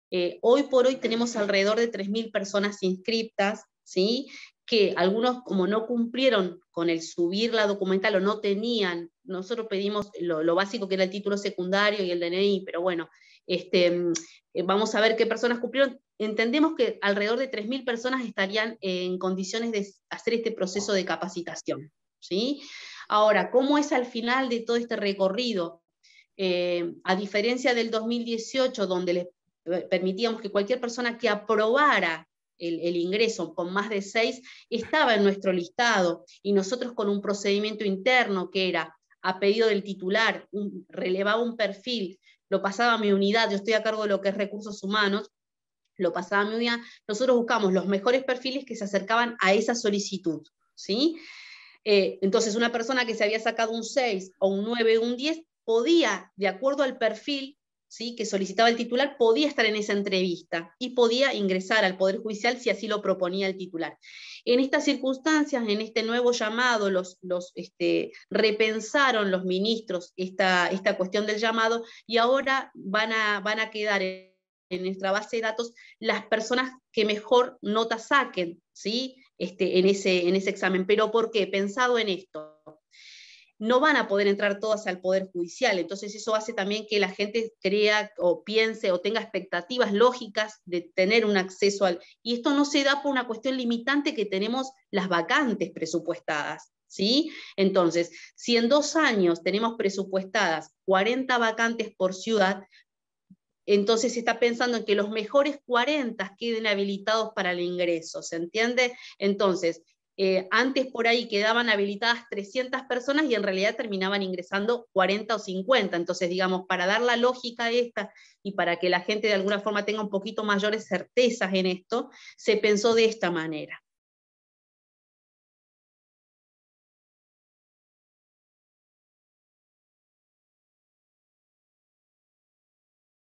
3MIL INTERESADOS PARA INGRESAR AL PODER JUDICIAL
3MIL INTERESADOS PARA INGRESAR AL PODER JUDICIAL

Hay 40 vacantes, los cuales serán para los mejores puntajes de las evaluaciones.